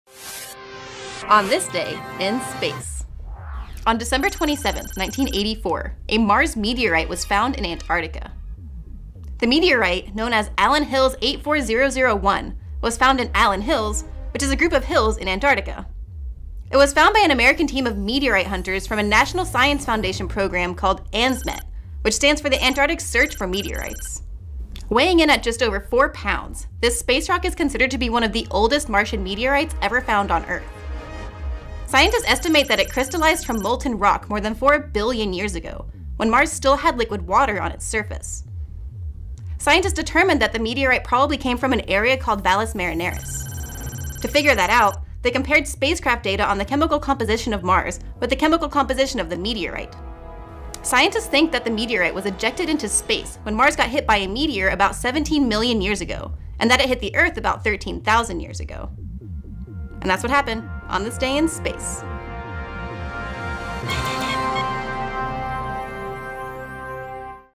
Mars meteorite found in Antarctica - On This Day In Space | Dec. 27
On Dec. 27, 1984, a Mars meteorite was found in Antarctica. -- ‘On This Day in Space’ Video Series on Space.com

The meteorite, known as Allan Hills 84001, was found in Allan Hills, which is a group of hills in Antarctica. It was found by an American team of meteorite hunters from a National Science Foundation program called ANSMET, which stands for the Antarctic Search for Meteorites.

Weighing in at just over 4 lbs., this space rock is considered to be one of the oldest Martian meteorites ever found on Earth. Scientists estimate that it crystallized from molten rock more than 4 billion years ago, when Mars still had liquid water on its surface.

Scientists determined that the meteorite probably came from an area called Valles Marineris. To figure that out, they compared spacecraft data on the chemical composition of Mars with the chemical composition of the meteorite.

Scientists think that the meteorite was ejected into space when Mars got hit by a meteor about 17 million years ago, and that it hit the Earth about 13,000 years ago.